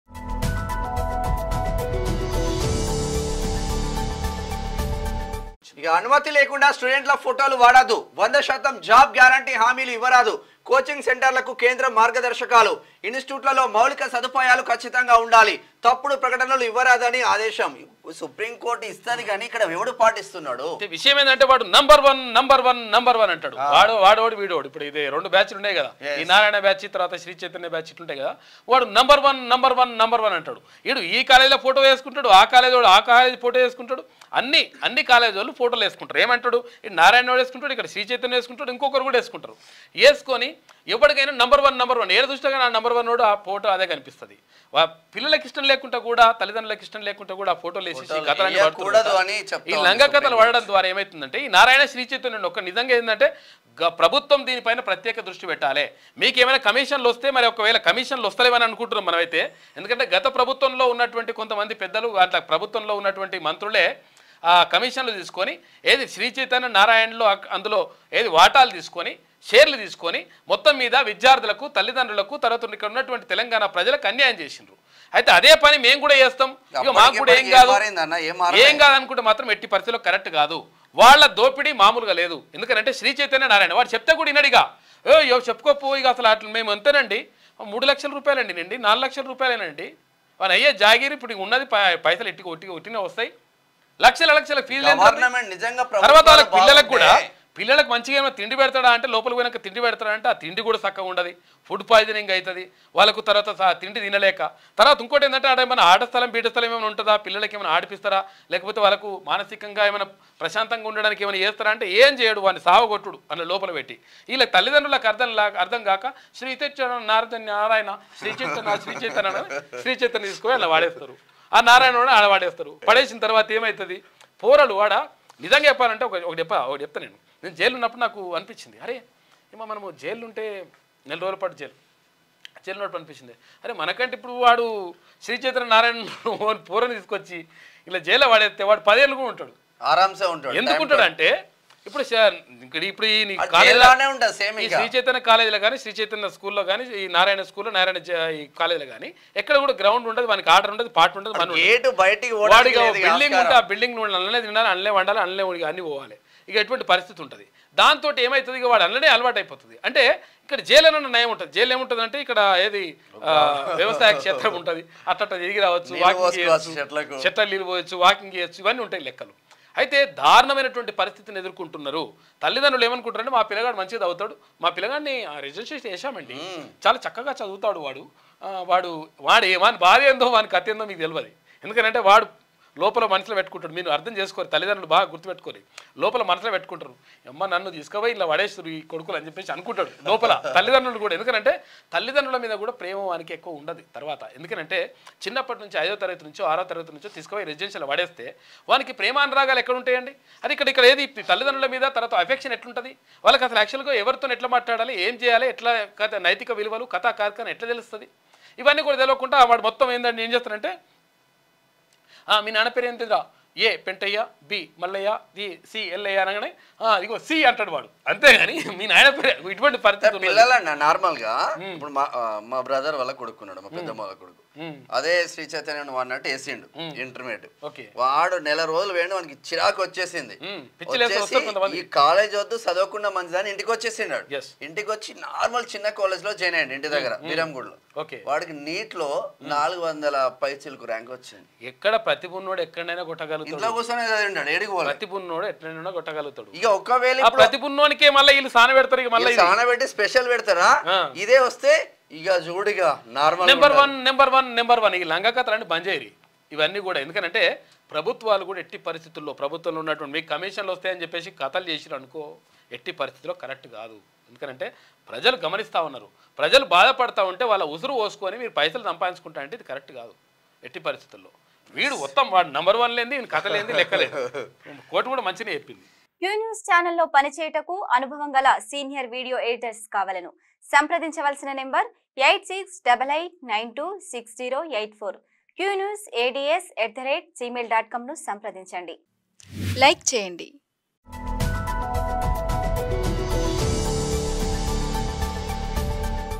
"Sudarshan Takes Strong Stand Against Sri Chaitanya and Narayana Colleges and Schools"| QnewsHD
Sudarshan Takes Strong Stand Against Sri Chaitanya and Narayana Colleges and Schools